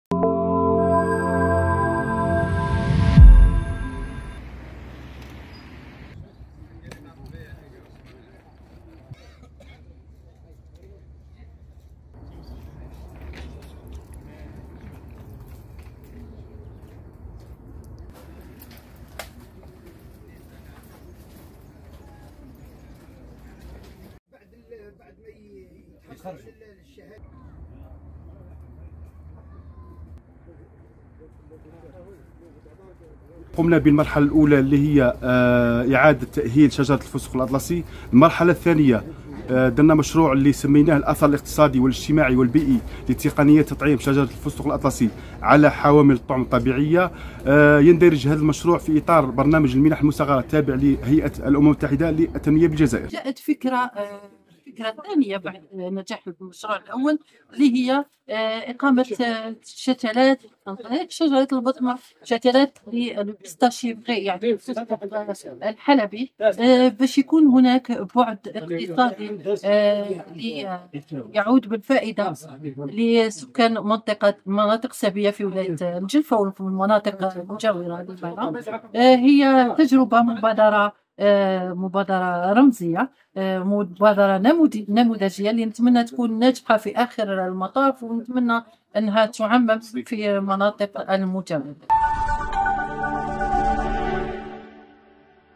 الجلفة: إطلاق مبادرة جمعوية حول تقنيات تطعيم شجرة الفتسق على حوامل الطعم الطبيعية
الجلفة (وأج) - أطلق مؤخرا بولاية الجلفة، مشروع جمعوي حول تقنيات تطعيم شجرة الفستق على حوامل الطعم الطبيعية، حسبما علم من القائمين على المنظمة الوطنية للشباب ذوي الكفاءات العلملية والمهنية من أجل الجزائر صاحبة هذه المبادرة .